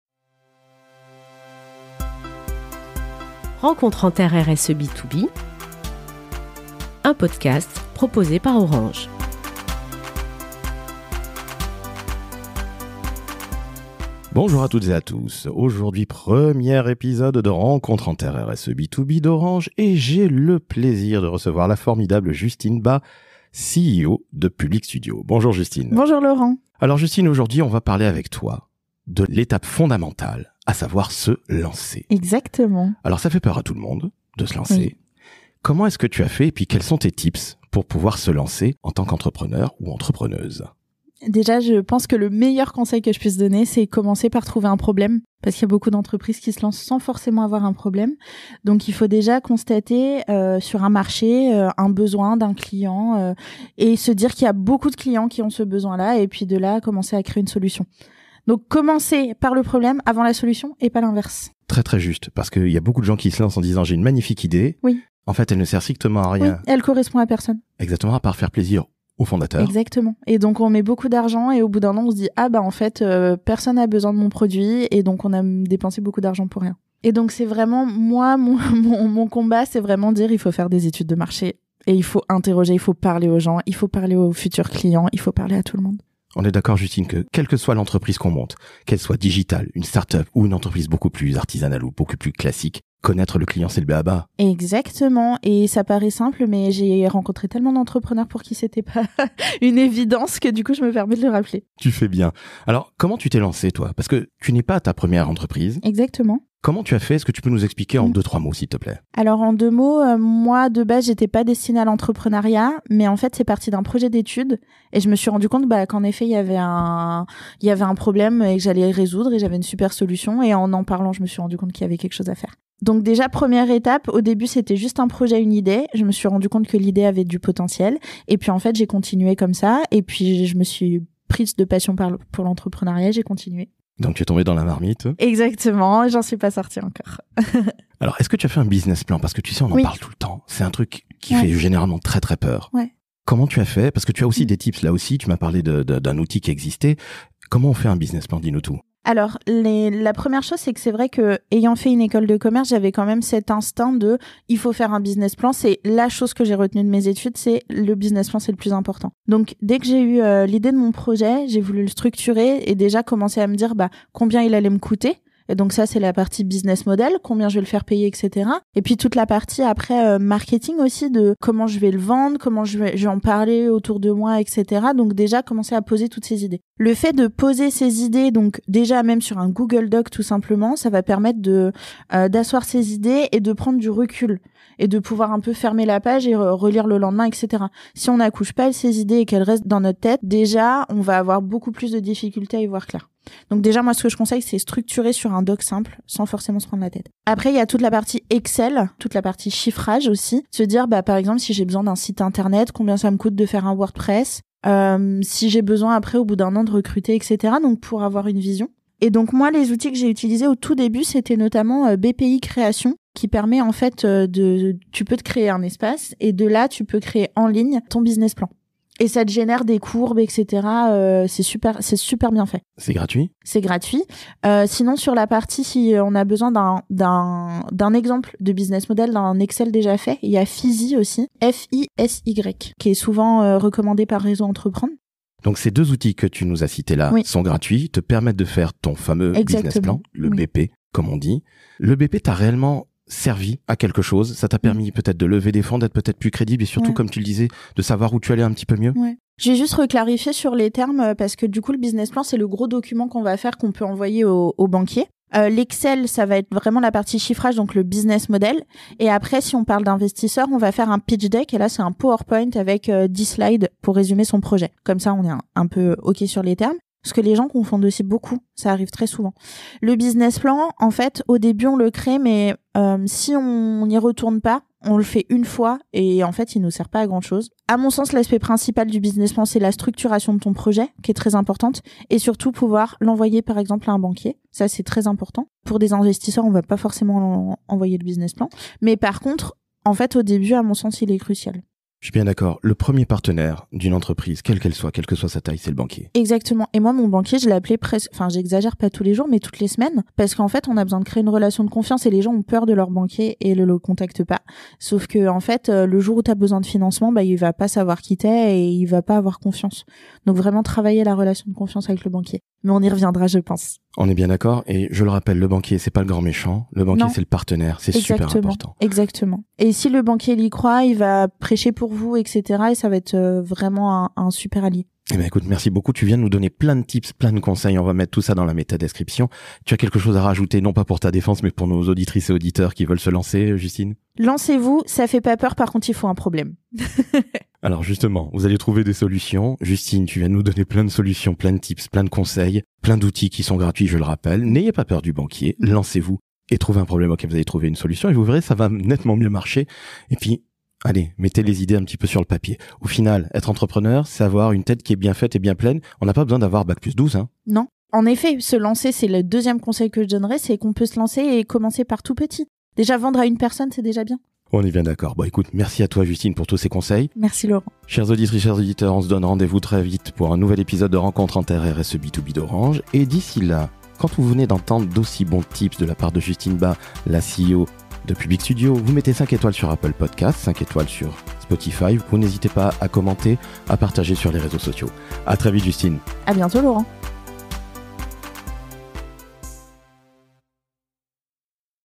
Rencontres en Terres RSE B2B d'Orange Lancer son Entreprise avec Justine BA Ep 01
Lancer son entreprise fait souvent peur. C'est pourquoi Justine Ba, CEO de Public Studio, vous donne ses conseils pour créer votre business.

Conseil #1 : trouver un problème et créer la solution qui y répond

Faire une étude de marché vous aidera à mieux comprendre les besoins de votre future clientèle et à vendre vos produits ou services. S'il vous plait, évitez de créer votre solution avant de trouver un problème à résoudre. Le cimetière de l'entrepreneuriat est plein de ces solutions inutiles...

Conseil #2 : faire un business plan

Le business plan permet de structurer votre projet et de prendre du recul.
Vous pouvez ainsi chiffrer vos coûts de structure, évaluer vos besoins marketing, comme la création d'un site Internet, ou encore les ressources nécessaires pour vous lancer : bureau, recrutement, ordinateurs, etc.

Sachez qu'il existe 2 outils GRATUITS pour réaliser votre business : BPI Création  et Fisy.

Pour rappel, il ne faut pas confondre le business plan, que vous envoyez à votre premier partenaire qu'est le banquier, et le pitch deck, beaucoup plus concis, que vous transmettez aux investisseurs.

Autre point essentiel : n'ayez pas peur de votre banquier. Il peut s'avérer être un excellent allié pour votre entreprise. Il est donc conseillé d'entretenir les bonnes relations avec lui !

Conseil #3 : commencer petit

Lancer son business, c'est vendre. Et, croyez-en l'expérience de Justine, tous les entrepreneurs se souviennent de leur première vente et de leur premier client !

A très vite pour un nouvel épisode de Rencontres en Terres RSE B2B d'Orange.

D'ici là, n'oubliez pas de noter 5 étoiles sur Apple Podcasts et Spotify.

Enfin, n'hésitez pas à partager cet épisode si vous souhaitez vous lancer dans le monde de l'entrepreneuriat.